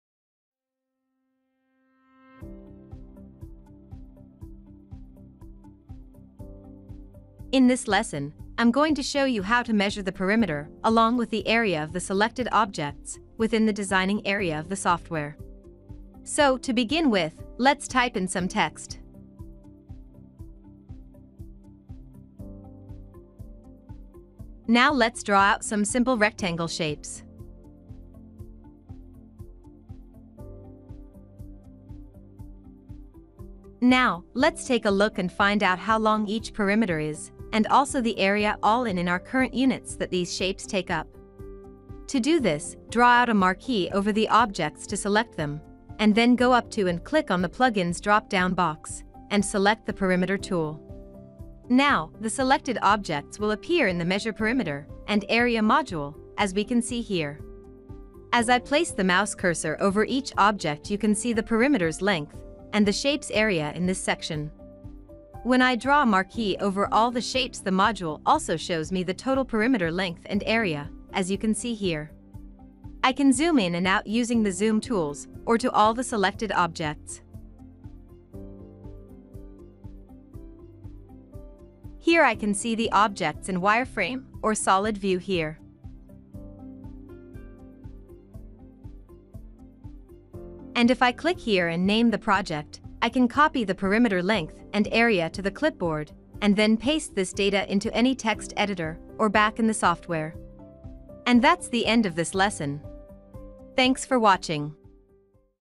Measure and Area Module in VinylMaster PRO V5 and all higher levels
A lesson on how to use the Measure and Area Module in VinylMaster V5. Available in VinylMaster PRO V5 and all higher levels.